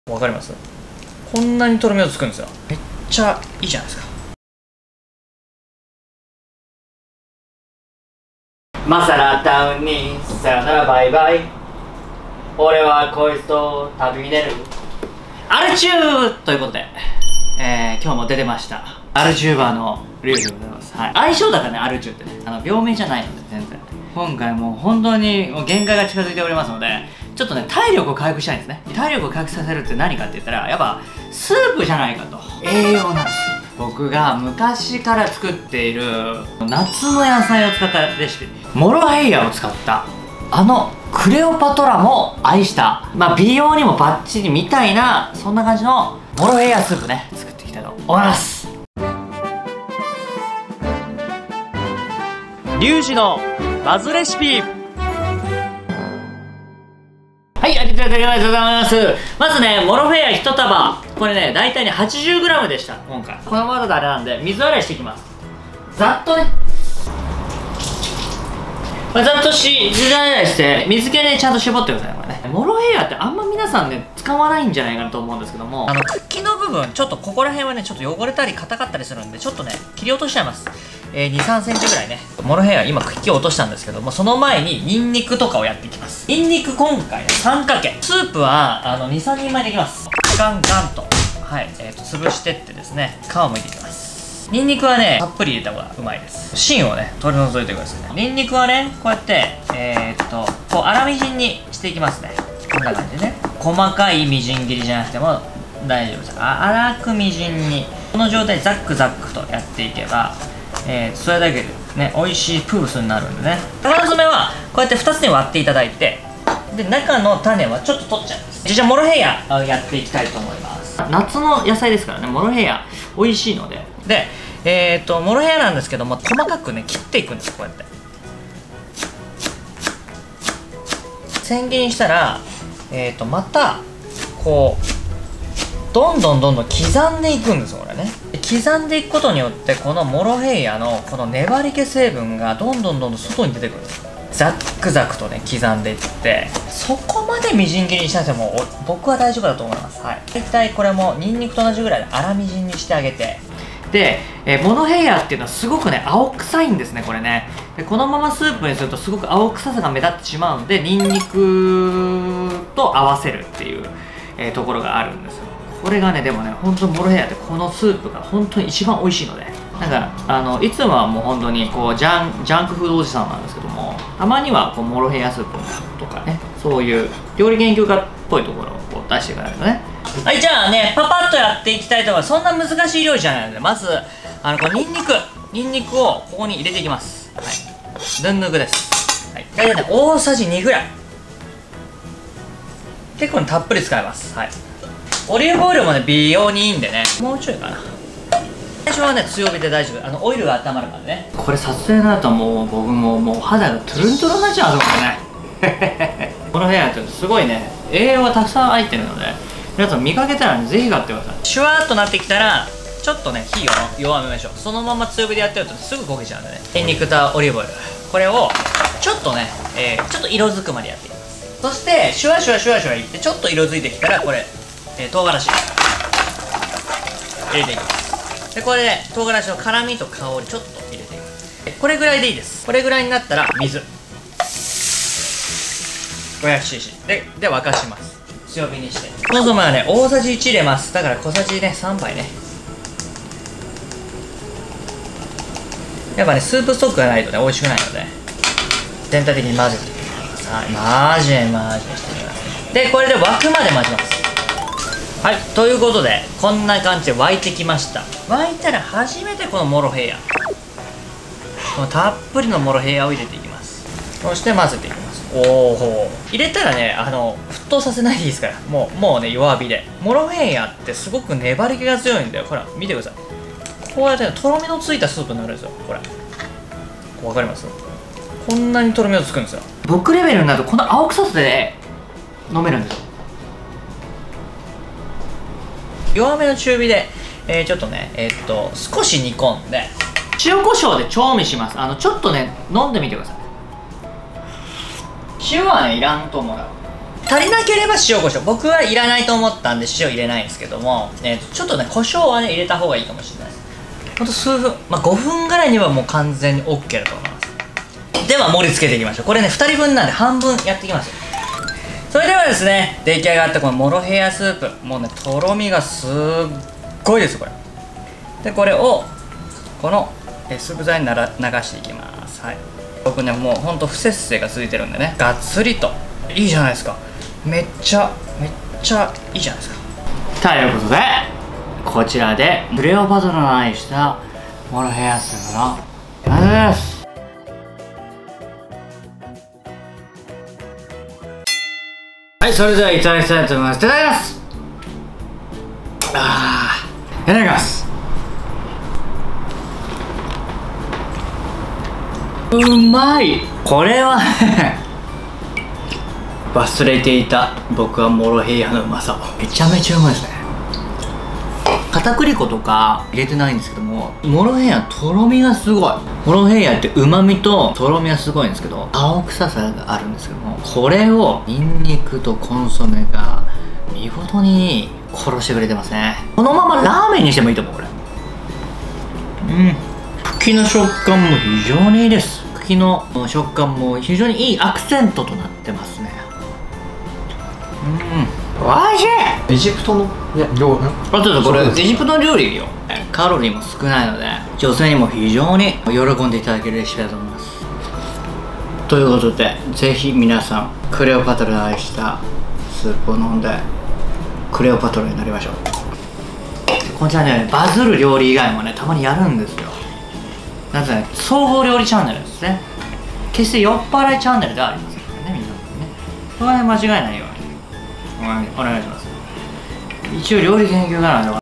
絶世の美女も愛した神栄養の神痩せ飯【モロヘイヤスープ】
【モロヘイヤスープ】

★今回のレシピはこちら↓
モロヘイヤ　1束
水　500ml
コンソメ　大さじ1
塩　適量
コショウ　適量

フリーBGM
DOVA-SYNDROME